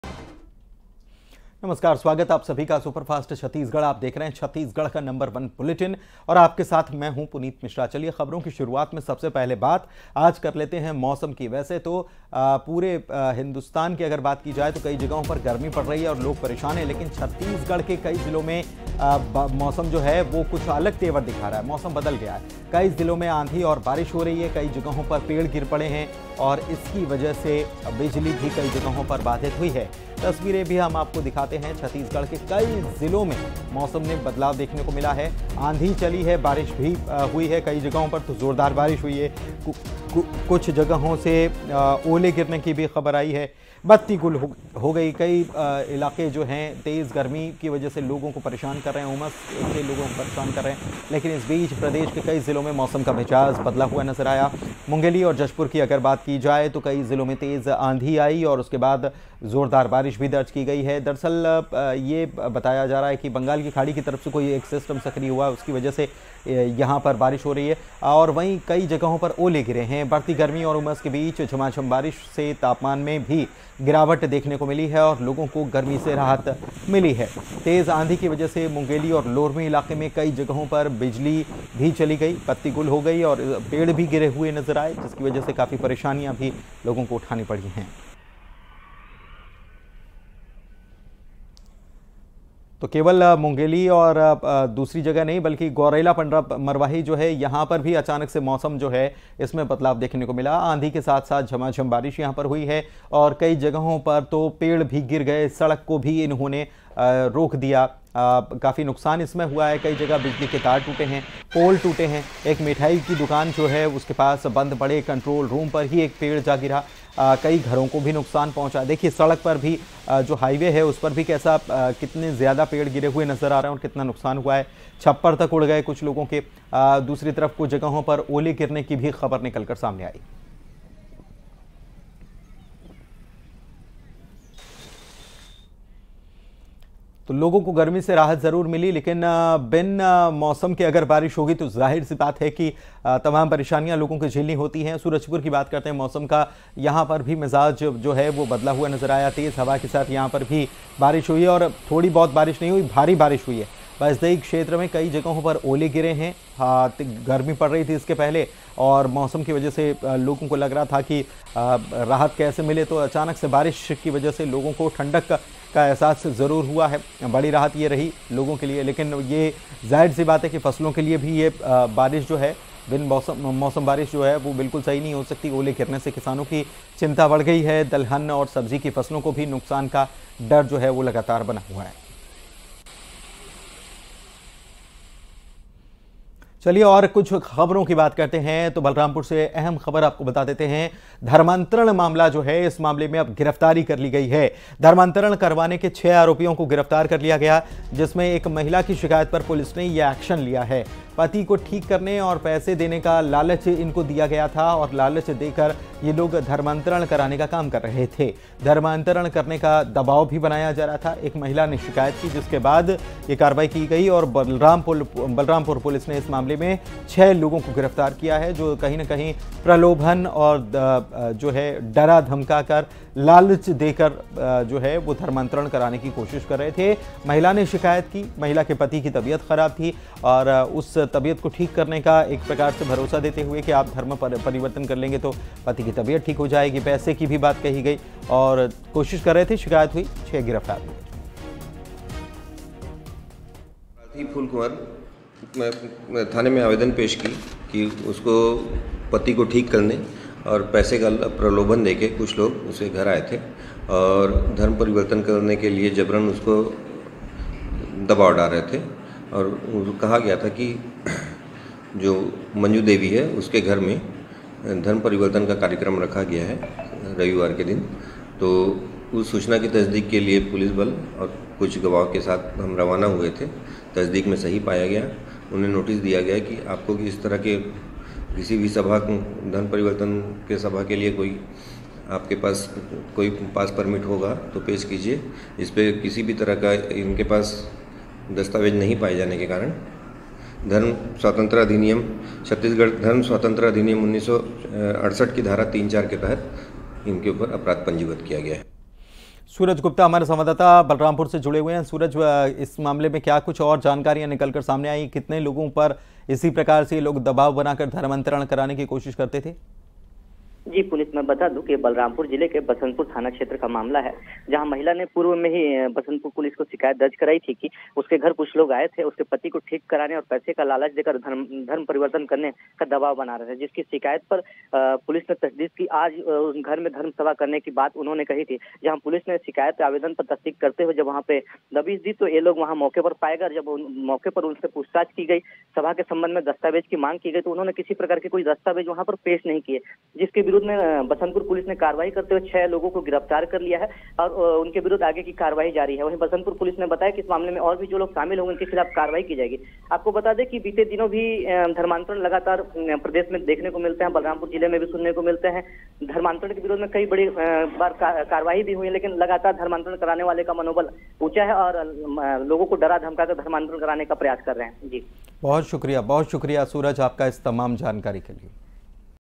Chhattisgarh के कई जिलों में आंधी और बारिश | Breaking News | Chhattisgarh rain | Weather News
Chhattisgarh के कई जिलों में आंधी और बारिश | Breaking News | Chhattisgarh rain | News Weather News

खबर है कि Pendra में गरज चमक के साथ हुई बारिश, कहीं भयंकर आंधी से पेड़ टूट गए तो कहीं तेज बारिश हुई